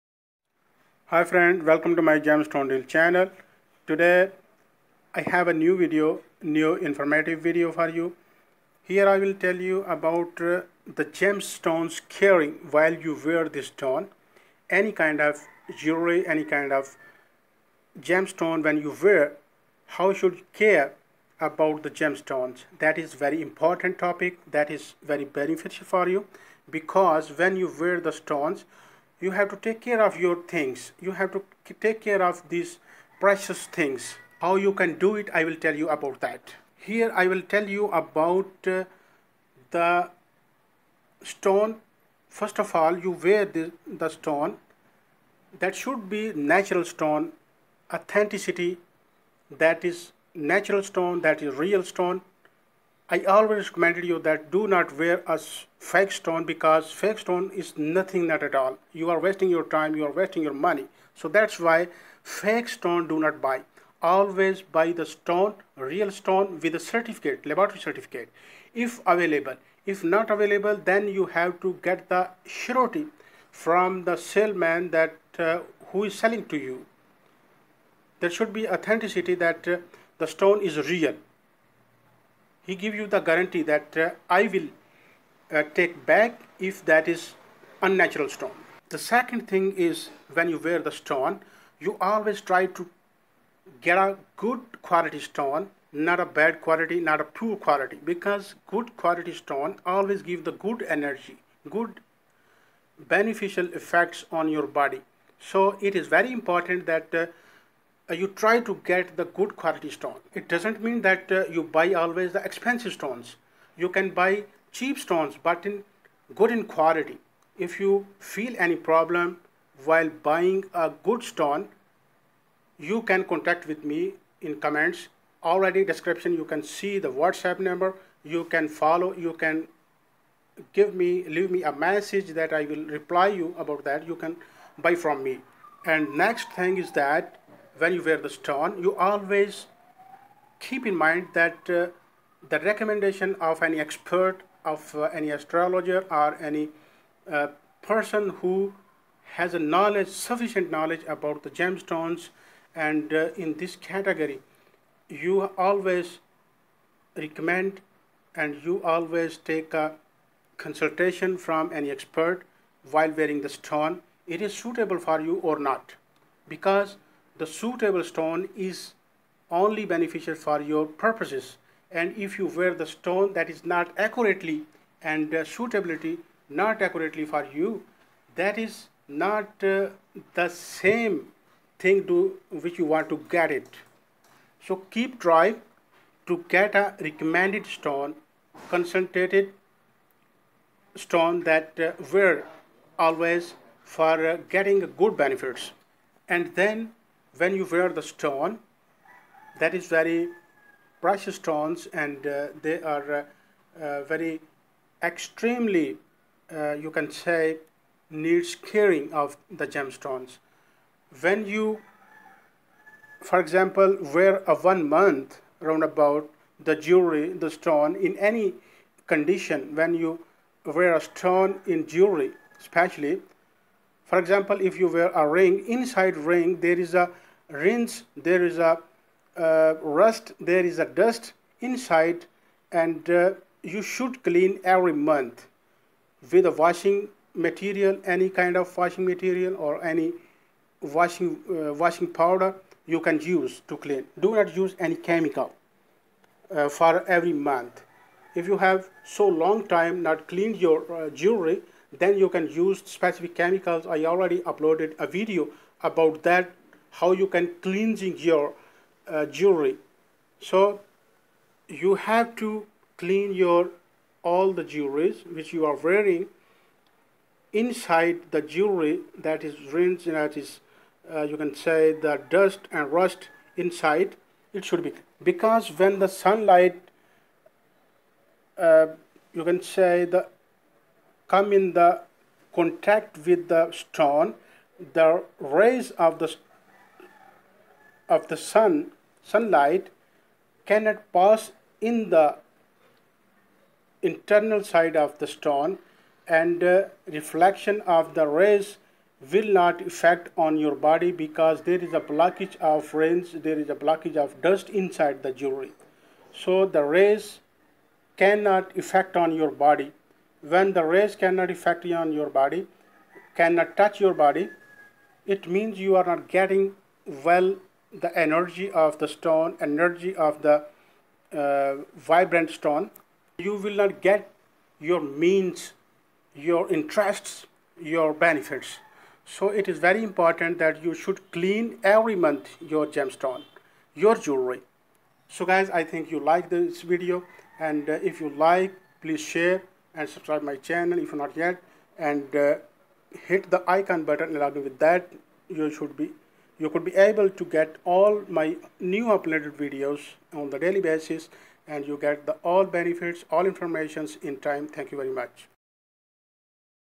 Amazing way to Caring your stones for more Benefits...By
Amazing way to look after your stones for more Benefits.

How You can Care of Your Gems and Jewellry for more Benefits.?

A guide describing frequently encountered gem treatment processes, their detection, and special care tips for treated gems.

 Caring for your therapeutic gemstones and gems is one of the most important things you can do to keep them working safely and effectively.
Our gemstone care guide covers many popular gemstone cleaining method.
Learn how best to maintain your gemstone jewelry.
Once you have acquired, inherited, or otherwise obtained a desirable gemstone, you must take care of it properly.
 Minerals are some of the hardest substances.

This video is very helpful and Beneficial.
 I wish you the best of luck.

More we will discuss in this video.